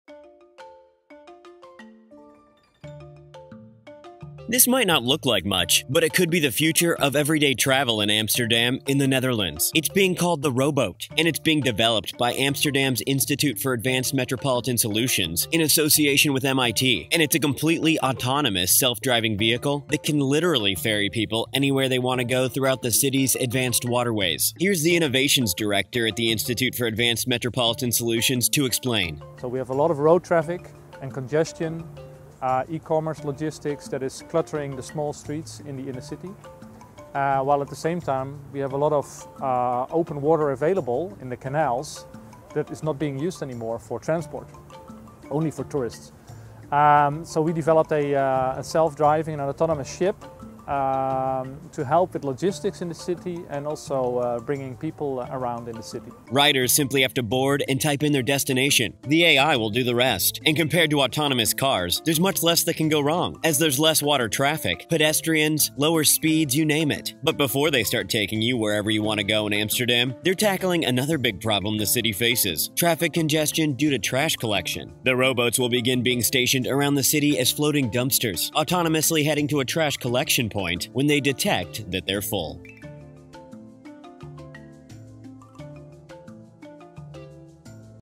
This Self-Driving ‘Roboat’ Solves Two Problems at Once in Amsterdam
It’s a clever solution to two problems facing Amsterdam’s busy streets.